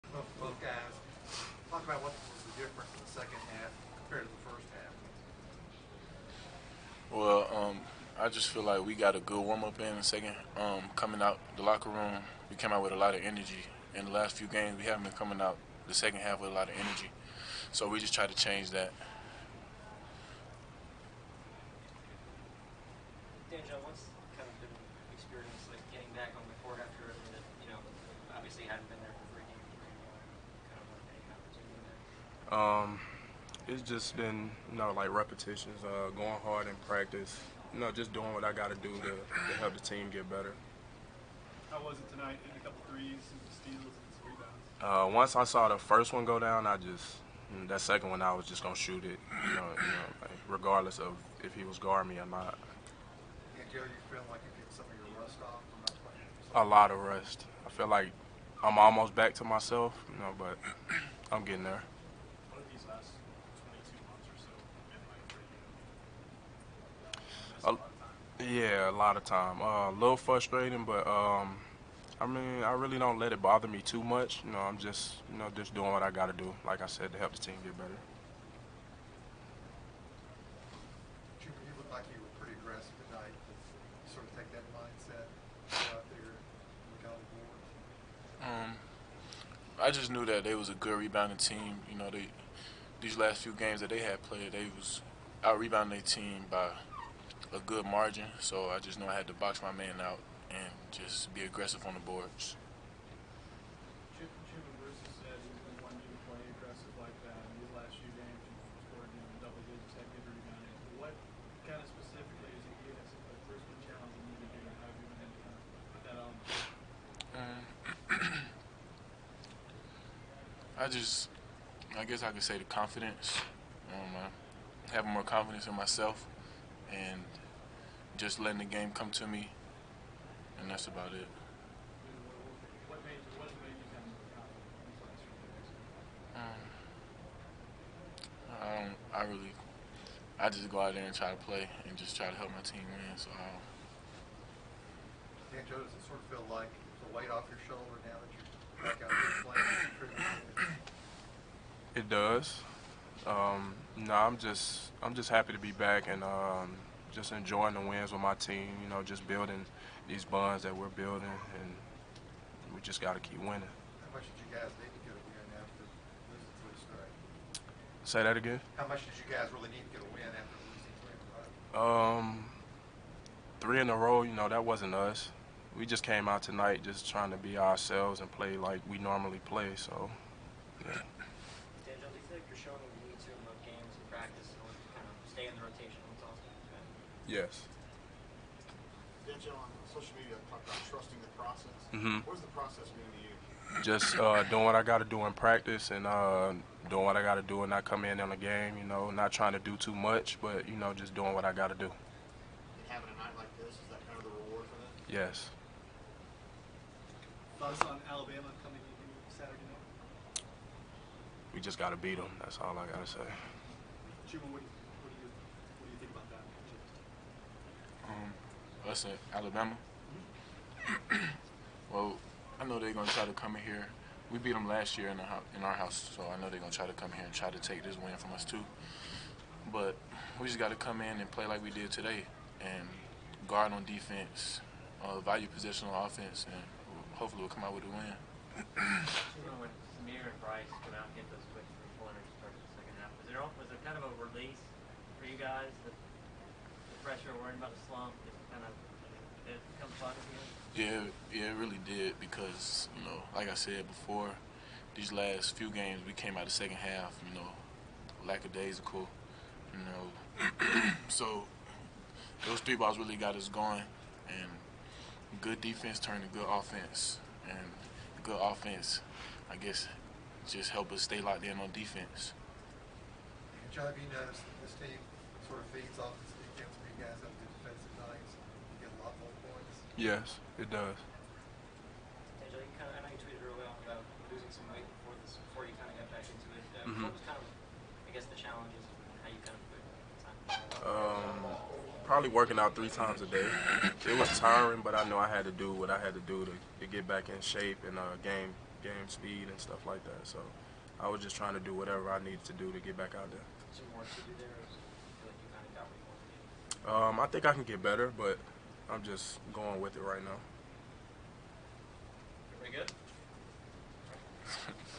Danjel Purifoy, Chuma Okeke discuss Auburn's win against Mizzou
Auburn players Danjel Purifoy and Chuma Okeke discuss the Tigers' 92-58 victory against Missouri. (Courtesy Auburn Athletics)